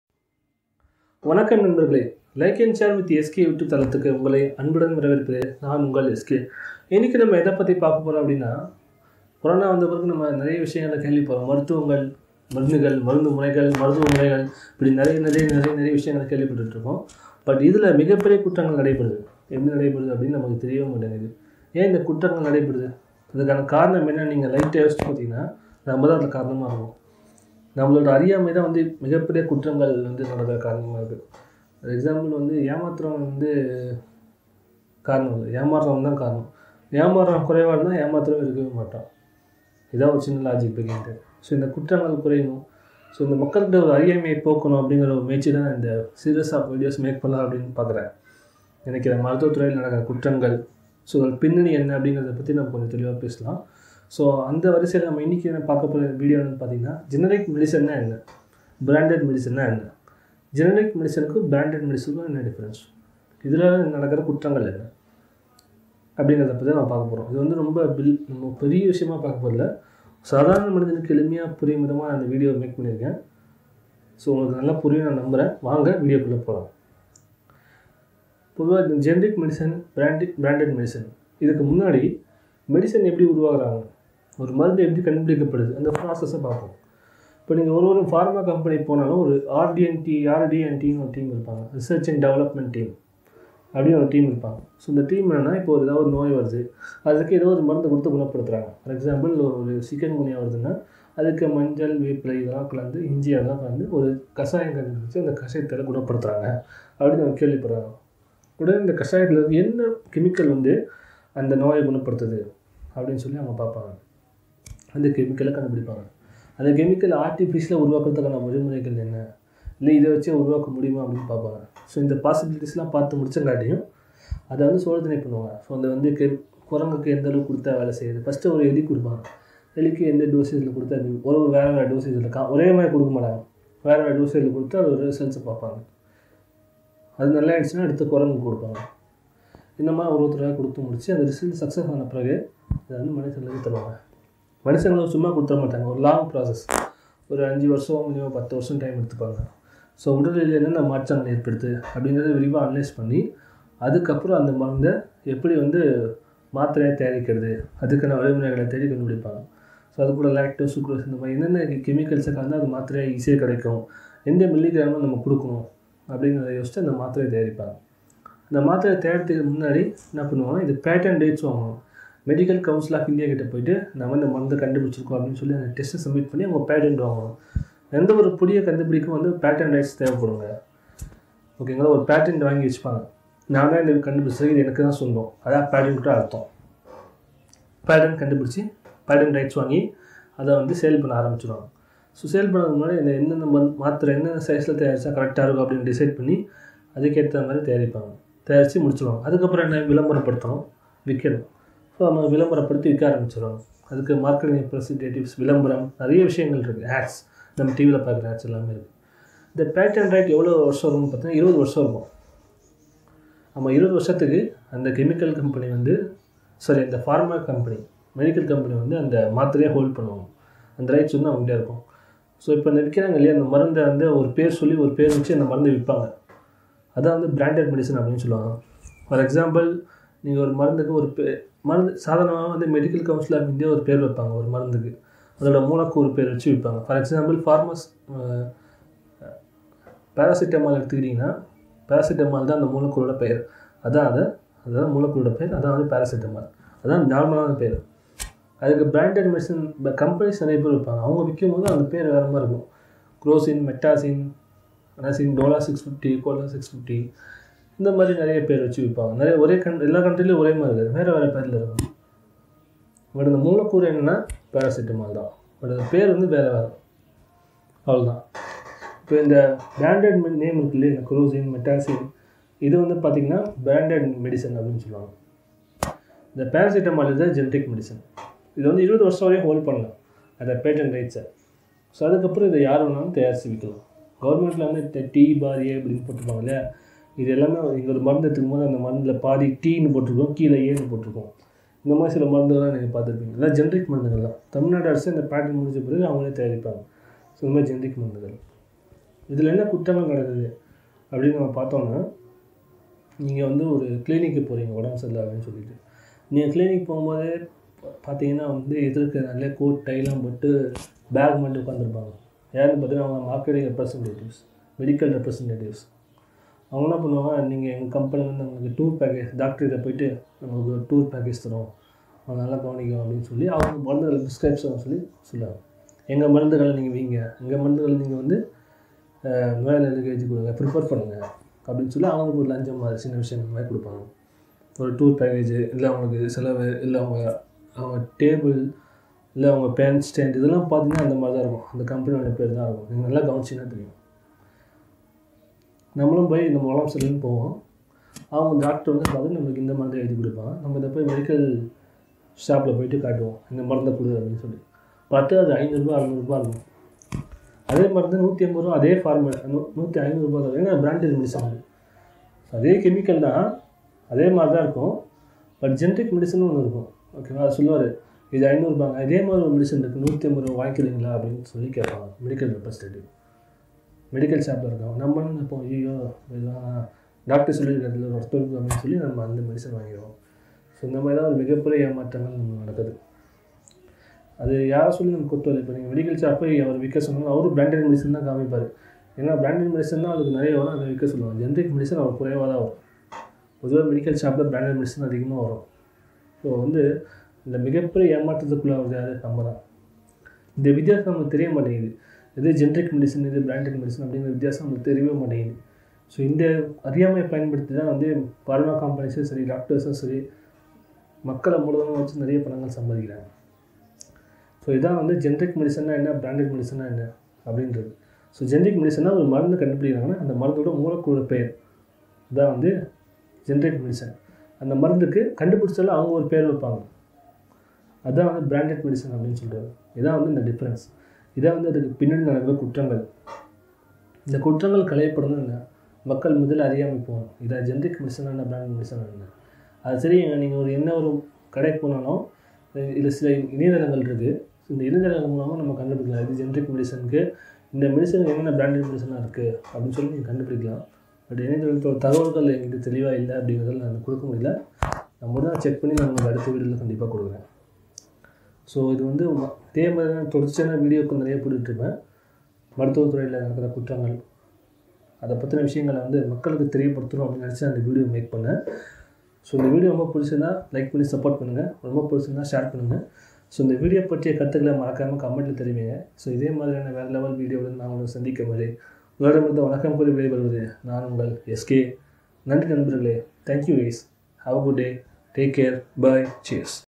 About Generic and Branded Medicines (must watch)
This video is about the generic and branded medicines and the differences,  and the issues involved in that.

Note: the images and videos are used under fair usage policy.